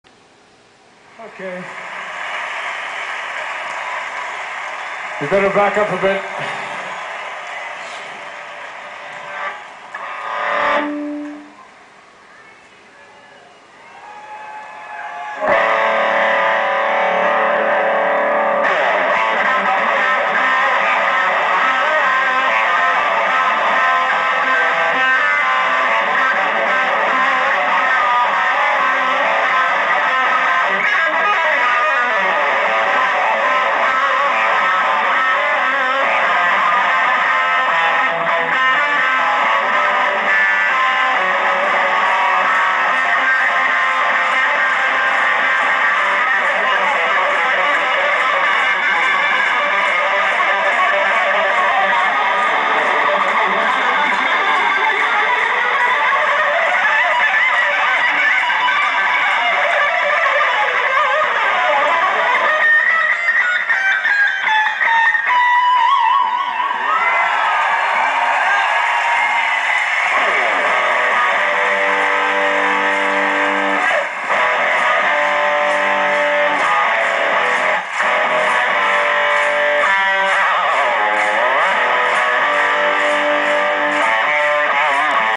Ian Crichton (Saga) - Guitar Solo !*!
Guitar solo of one of the best guitar playas on this planet! Great!